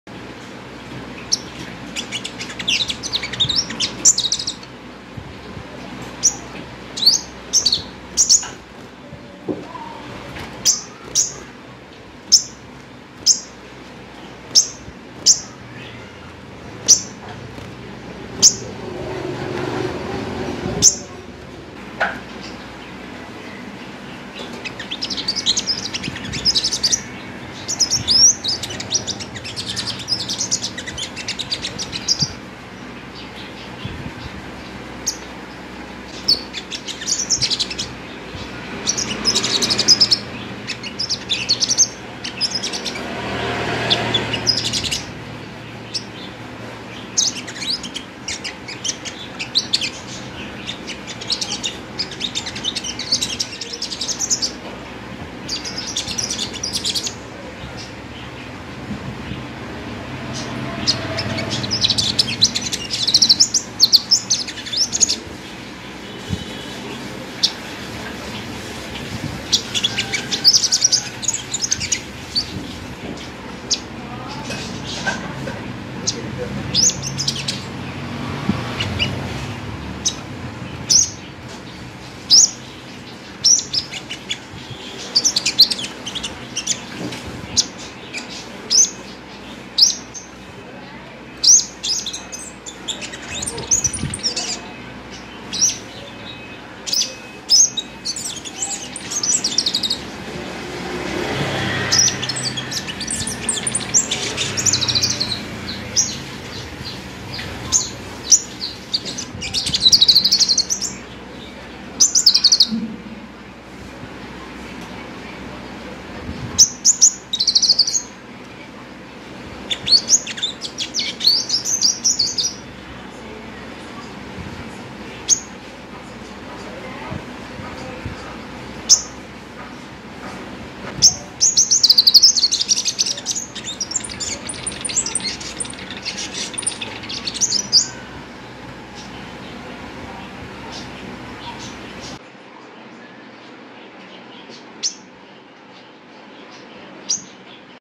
Tiếng hút mật 5 màu chuyền hót,líu.
Chim hút mật 5 màu nếu mình nuôi chơi nghe hót giải trí,theo cá nhân mình nếu nuôi được chim non,chim chuyền.dù thời gian lâu và cực nhưng sẽ rất dễ có một chú chim làm mình thỏa mãn.chim sẽ siêng líu lo và hót nhiều,còn dạn người thì điều đương nhiên phải không ạ.mình đã nuôi vài em lên biết được chút ít,chúc các bạn luôn thỏa mãn với niềm đam mê của mình nha!nếu muốn các bạn có thể đăng ký kênh để ủng hộ những video tiếp theo.cảm ơn các bạn đã xem video.